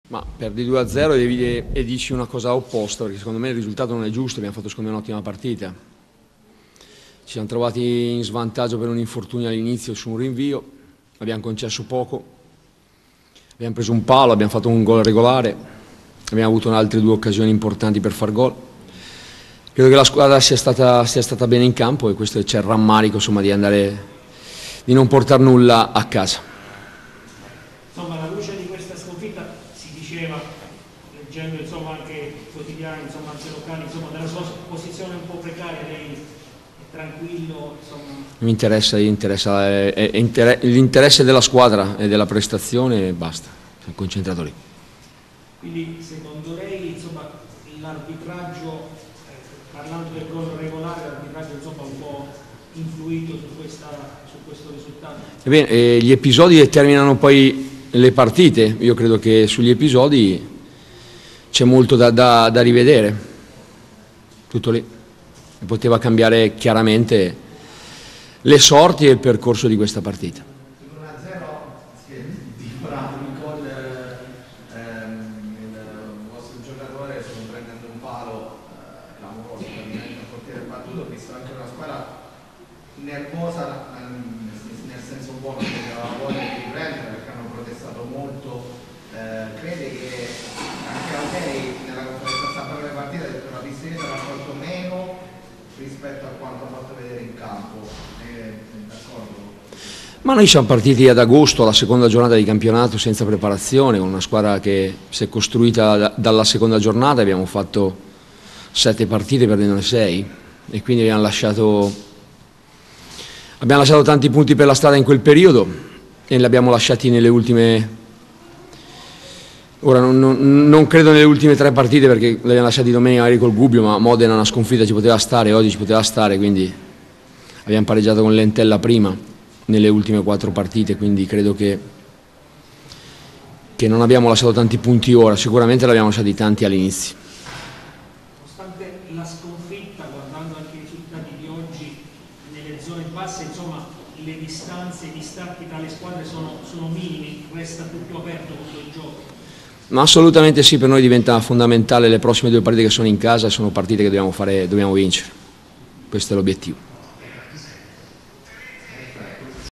Pescara - Pistoiese 2-0: David Sassarini
Conferenza post gara dell'allenatore della Pistoiese